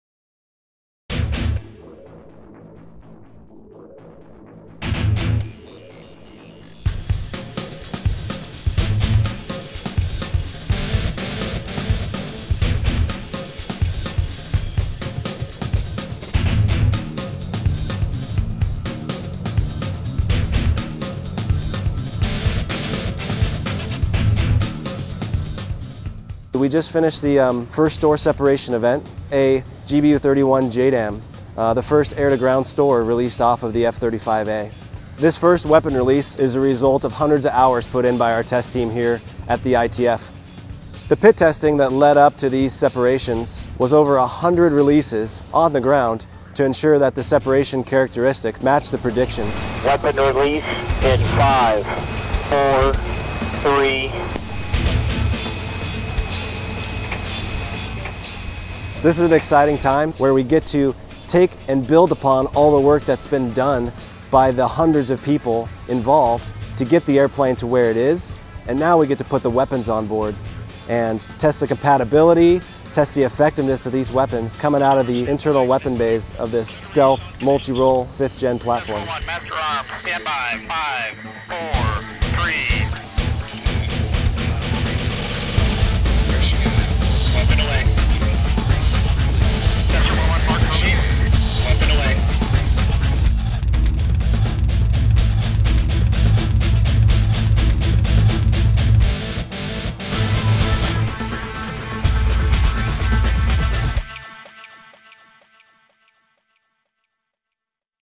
First Weapons Release From F-35A
An F-35A conventional take-off and landing (CTOL) aircraft completed the first in-flight weapons release this week.

The weapon was  a 2,000 pound GBU-31 BLU-109 Joint Direct Attack Munition (JDAM) dropped form the 5th Generation jet fighter.